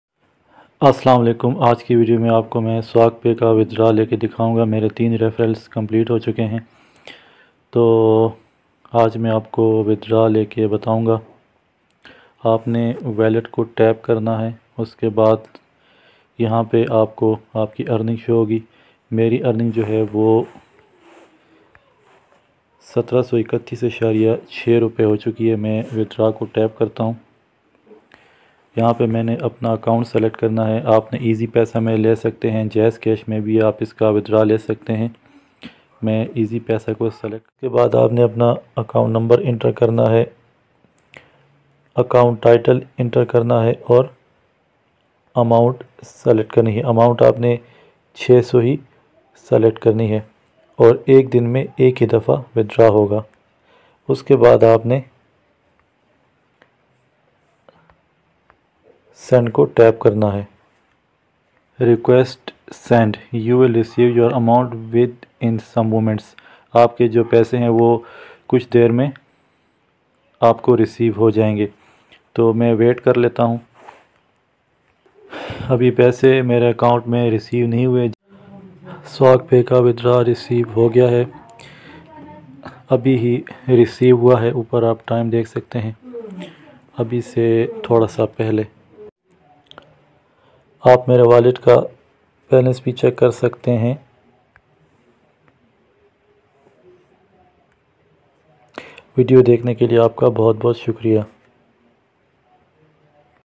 Swaggpay 600PKR Withdraw Proof|Swaggpay Earning Website|Technical Shahnavi
This video is about swaggpay earning withdraw proof. I withdraw 600pkr from swaggpay website today.

plz watch this video completely